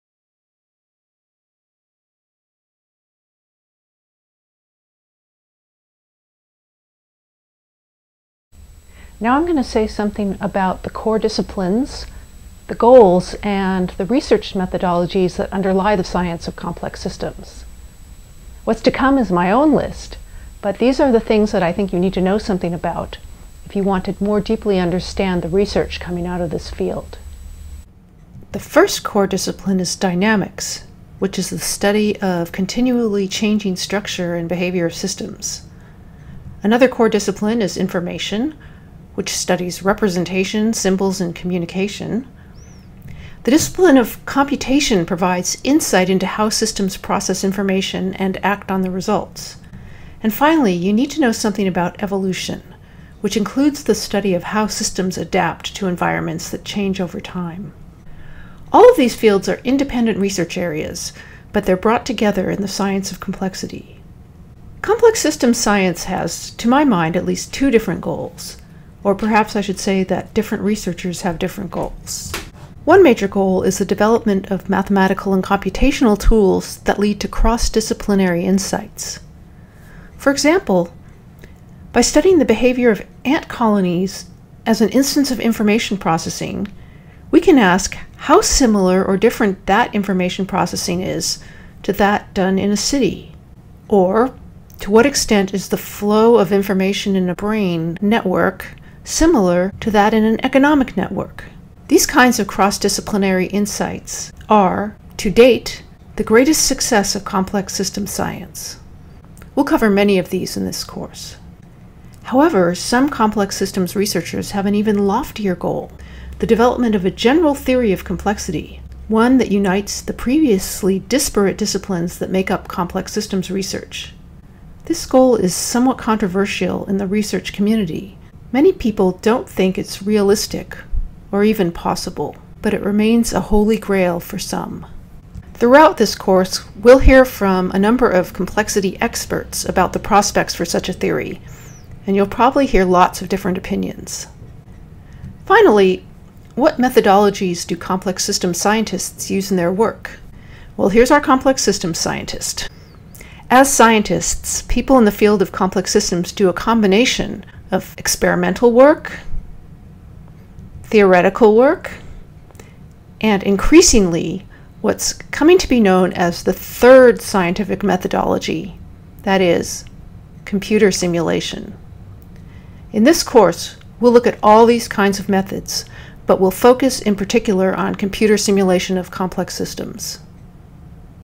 Introduction to Complexity: Core Goals, Disciplines, and Methodologies of the Sciences of Complexity
These are videos from the Introduction to Complexity course hosted on Complexity Explorer. You will learn about the tools used by scientists to understand complex systems. The topics you'll learn about include dynamics, chaos, fractals, information theory, self-organization, agent-based modeling, and networks. You’ll also get a sense of how these topics fit together to help explain how complexity arises and evolves in nature, society, and technology.